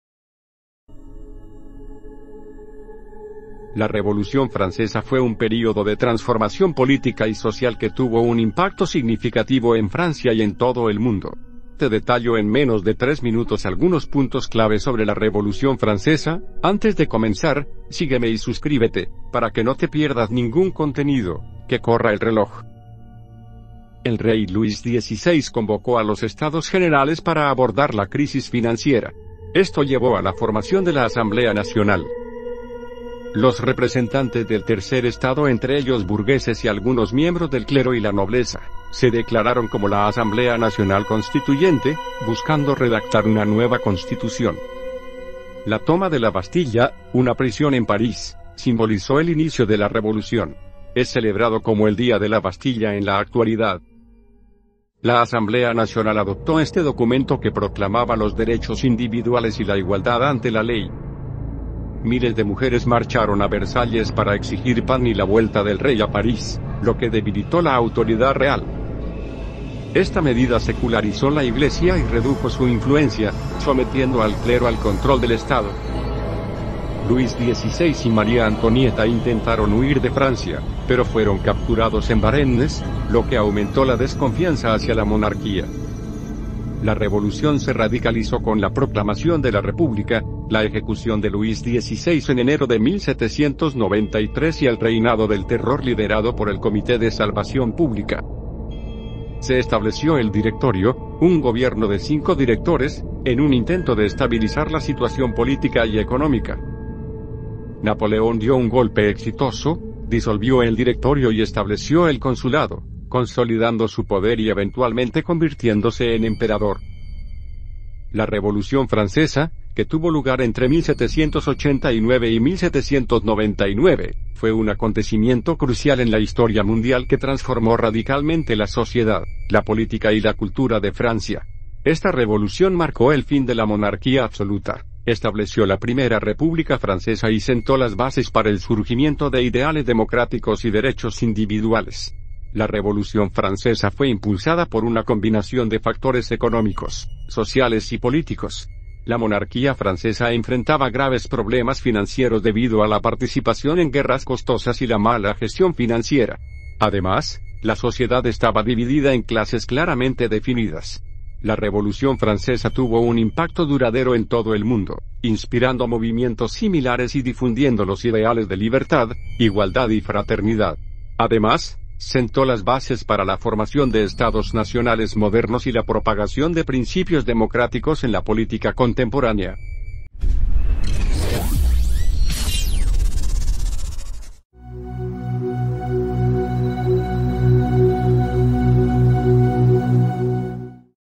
La Revolución Francesa en 3 minutos...!!!
La Revolución Francesa (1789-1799) fue un proceso transformador impulsado por crisis económicas, sociales y políticas en Francia. La monarquía enfrentaba dificultades financieras, y el Tercer Estado, cargado de impuestos, resentía los privilegios de la nobleza y el clero. La convocatoria de los Estados Generales en 1789 y la formación de la Asamblea Nacional Constituyente marcaron el inicio de la rebelión. La toma de la Bastilla y la Declaración de los Derechos del Hombre simbolizaron la lucha por libertad e igualdad. La revolución culminó con el ascenso de Napoleón Bonaparte, influyendo en la política moderna globalmente.